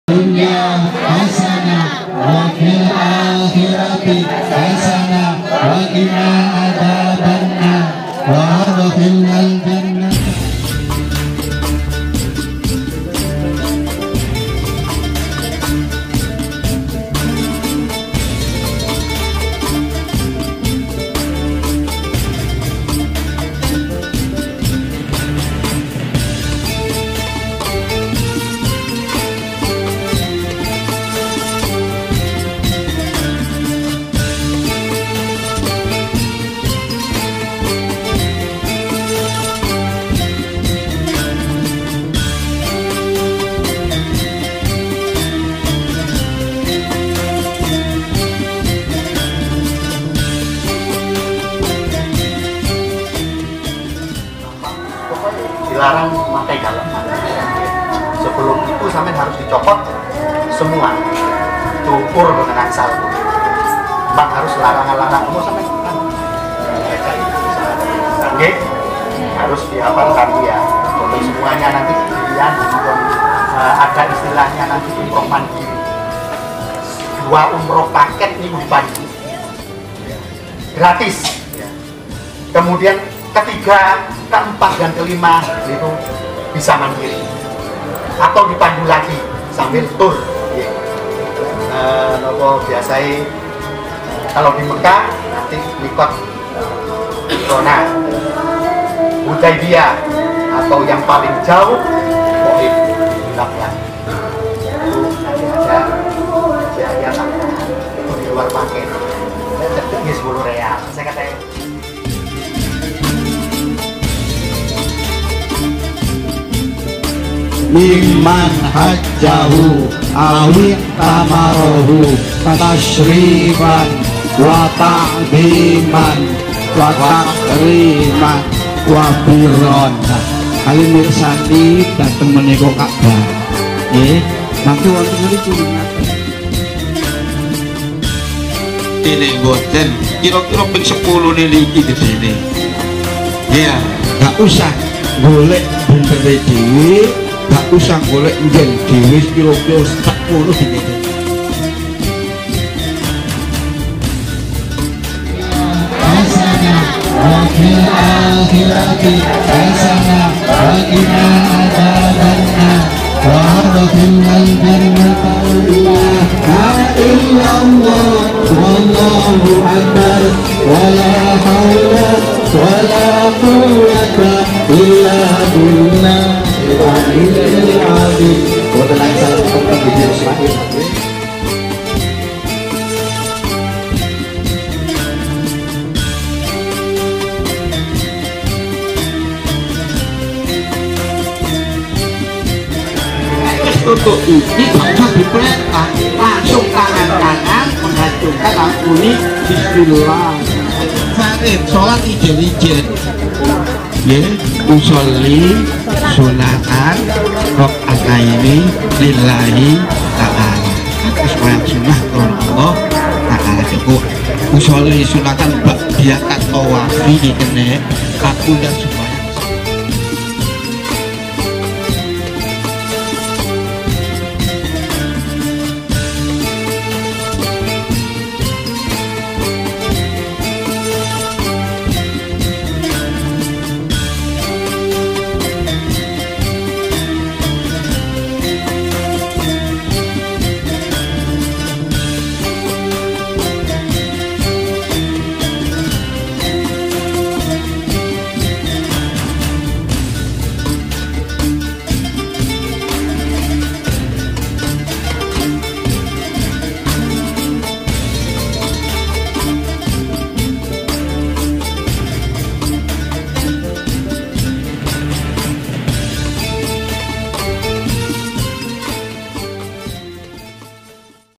MANASIK Sebelum berangkat umroh di bulan September 2022 | Jama'ah umroh Cahaya Indah NURUSH SHOBAH
MANASIK Sebelum berangkat umroh di bulan September 2022 | Jama'ah Haji Cahaya Indah NURUSH SHOBAH

MANASIK Pembekalan Sebelum Berangkat Umroh

Lokasi :

KEBUN KURMA PURWOSARI-PURWODADI "PASURUAN JAWA TIMUR"
Dalam Bimbingan Ust. Sumitro